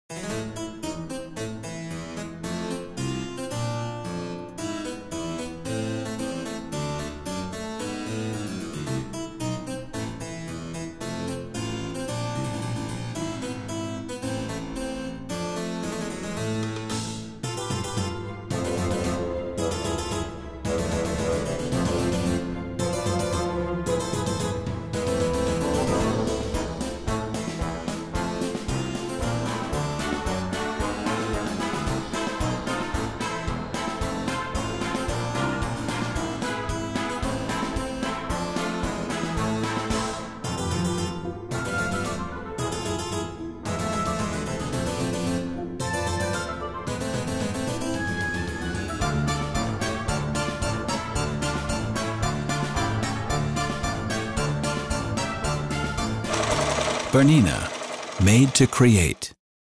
Minky Ghost in the Hoop
Designed as an in-the-hoop project, this adorable ghost can be created in no time; a great first embroidery project for beginners!